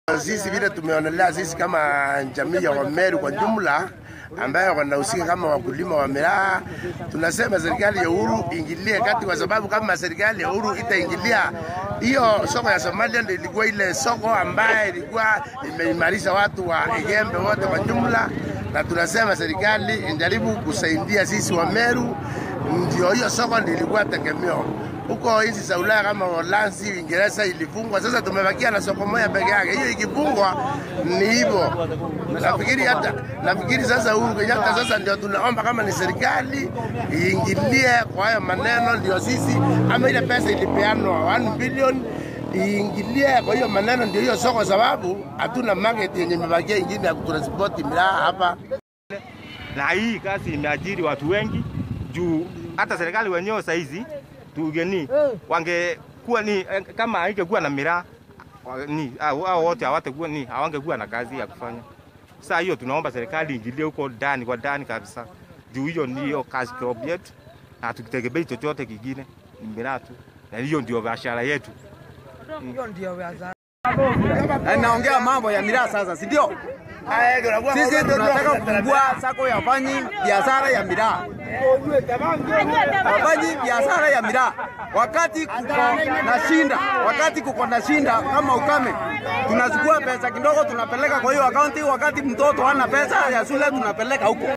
Miraa farmers now pointing fingers at the government
For many years, the Nyambene area has enjoyed booming business propelled by the sale of khat.
But now thousands of miraa farmers in Meru County are facing an uncertain future after the Somali government banned the importation of the crop.
They are now blaming the government for failing to lobby authorities against banning the produce.